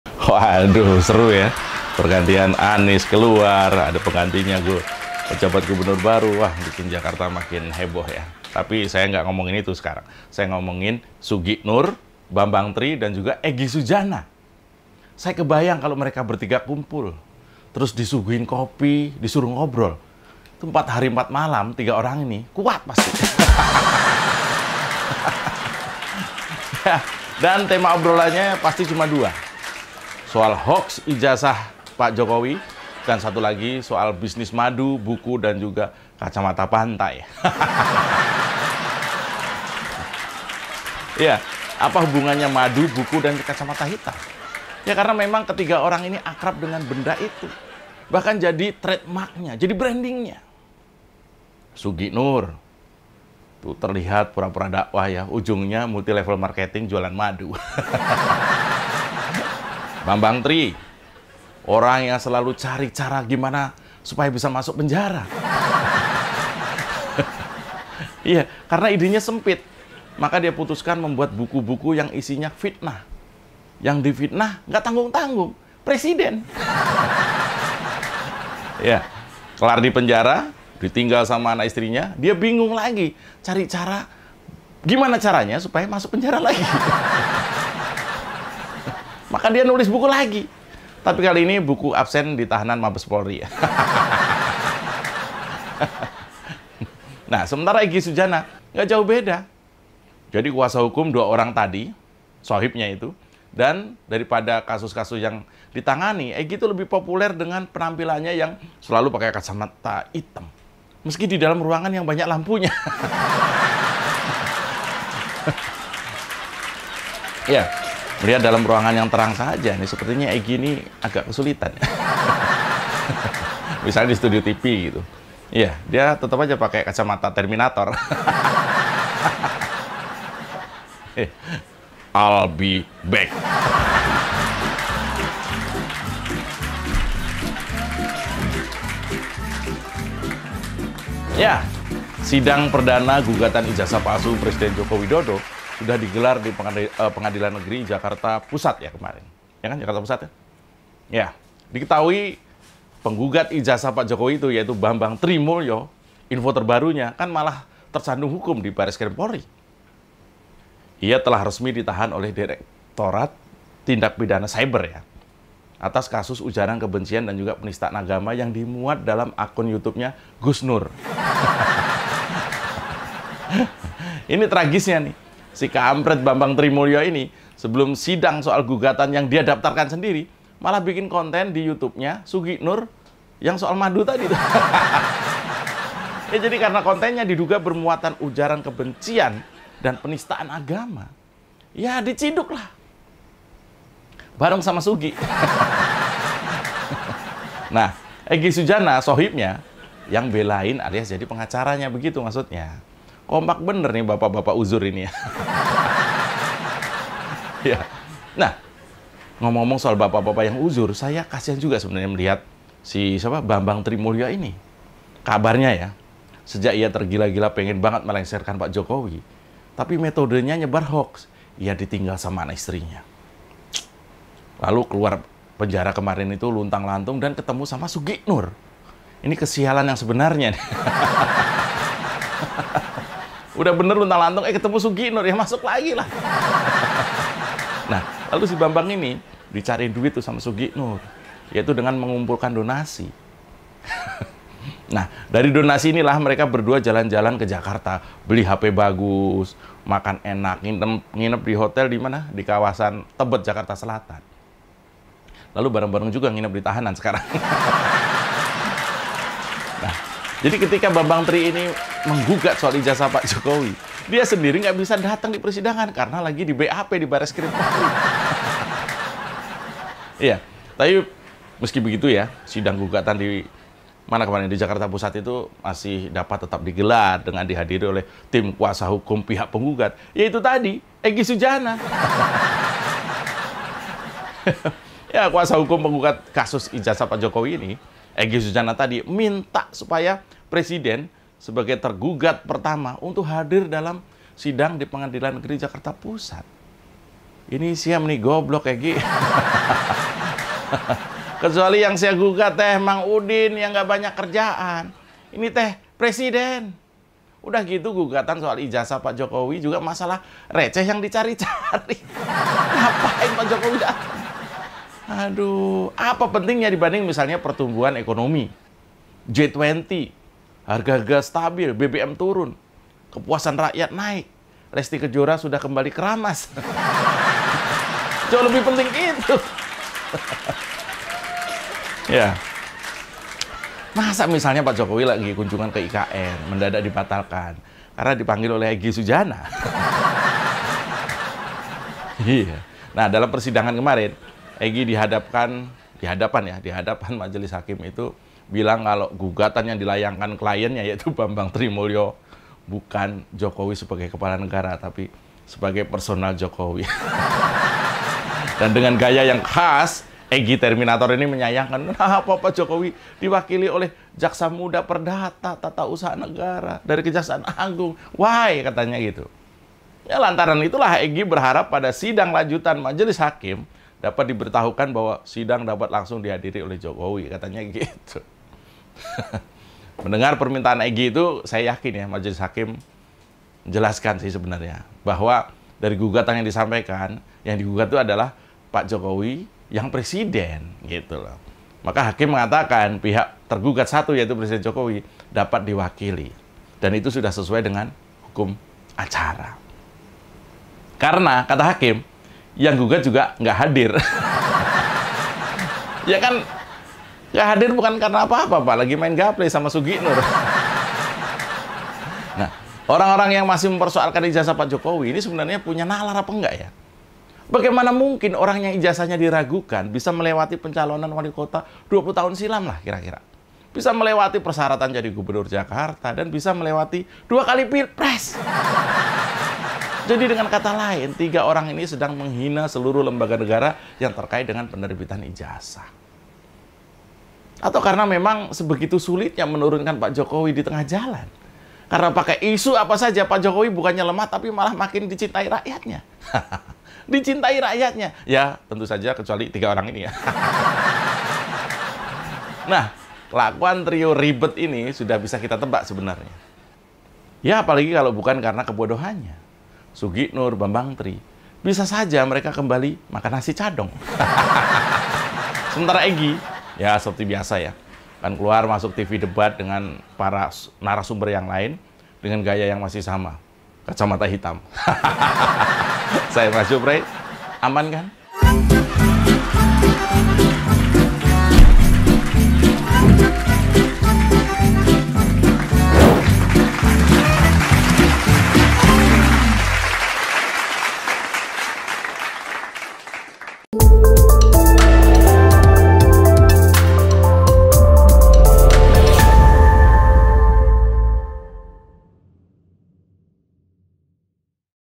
Mazdjo Pray | SUGIK NUR, BAMBANG TRI MULYO DITAHAN POLISI. EGGI SUDJANA GIMANA? | Mazdjo Show #63
Hoax ijasahpalsu yang gembar gemborkan sugiknur dan teman-temannya, eggisudjana jadi ribet sendiri!